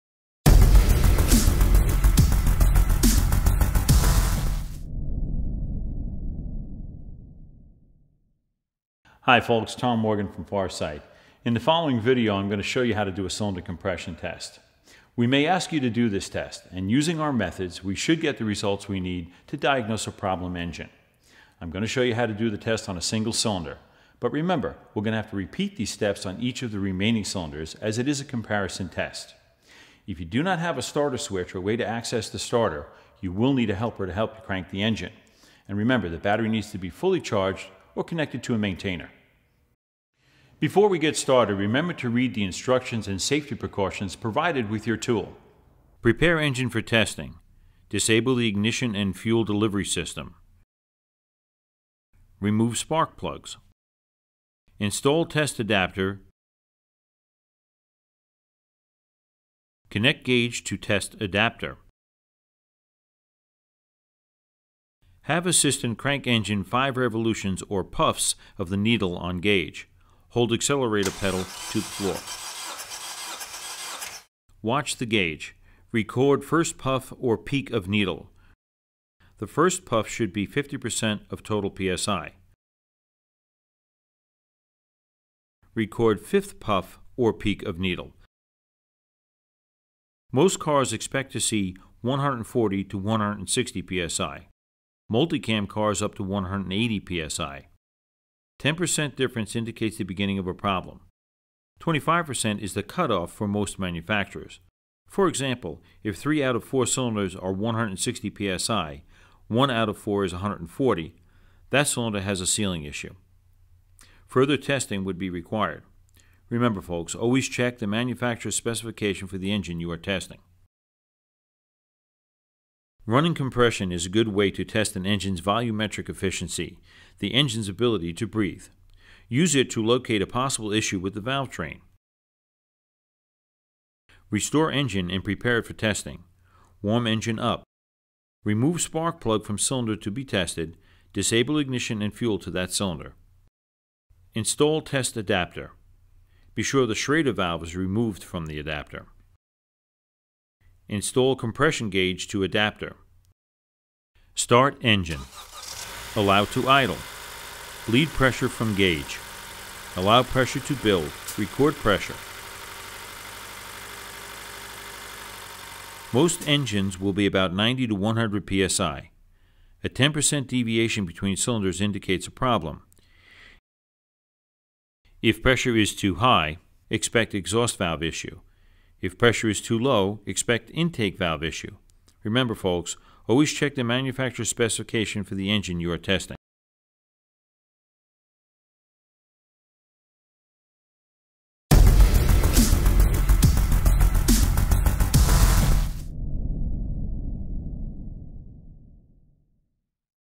How to Perform a Static & Dynamic Compression Test
In this video, Tom Morgan FARSIGHT Technical Support Specialist, demonstrates how to perform a static and dynamic compression test. He also reviews what to expect for nominal values and common causes when diagnosing using cylinder comparisons.